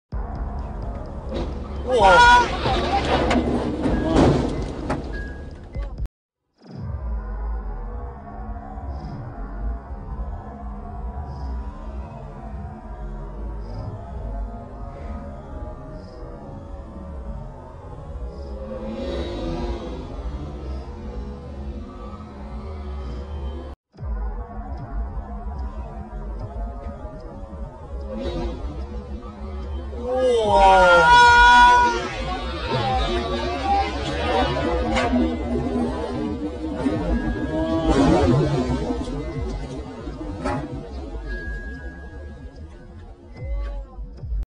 車Cam直擊｜牛池灣兩車相撞翻滾鏟上巴士站
下午4時許，龍翔道牛池灣村對開，一輛私家車與的士相撞，其間的士失控衝上行人路，衝破一堆建築材料後，再撞入村內一間售賣葛菜水的鐵皮舖頭，所幸事發時店舖並沒有營業，至於私家車剷上行人路巴士站。

據現場一輛正準備駛出龍翔道私家車的車Cam片段顯示，白色私家車左面車身與的士右邊車身相撞後，白色車隨即從左邊彈起四輪朝天，打轉翻滾一圈，衝向行人路後停下；至於的士右前方車胎爆破，撞向左方路邊建築物。

頭條日報